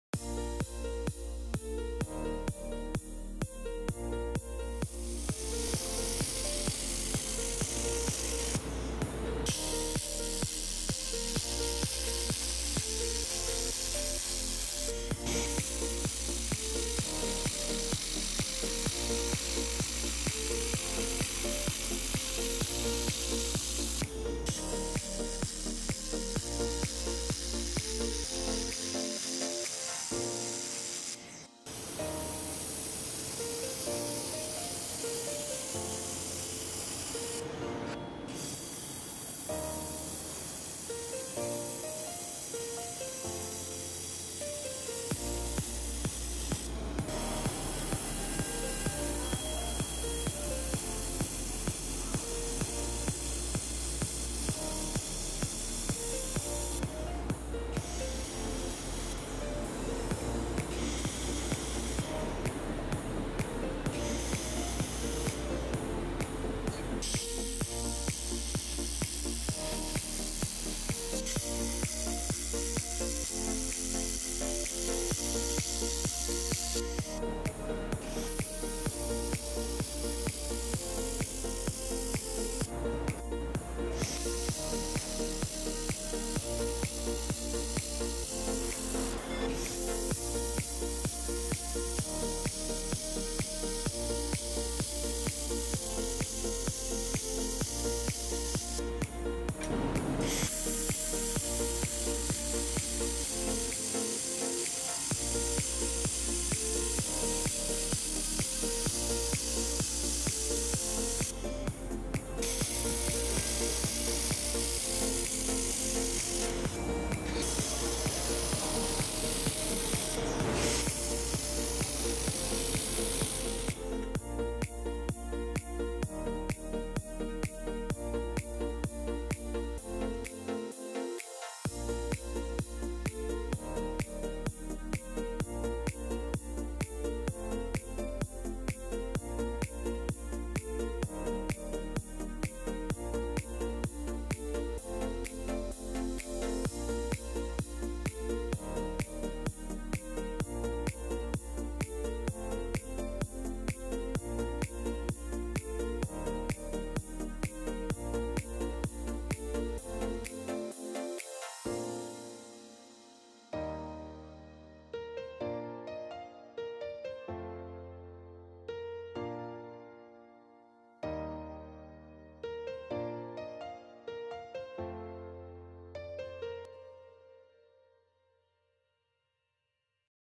1500W Handheld Laser Welder Welding Show
The handheld laser welding machine is a welding product developed and manufactured by SENFENG LEIMING LASER reached the domestic advanced level. This product is the preferred model in the metal material welding and processing industry.The machine can weld all kinds of metal. The welding seam is beautiful and firm, and the operation is simple, fast, flexible and low in processing cost.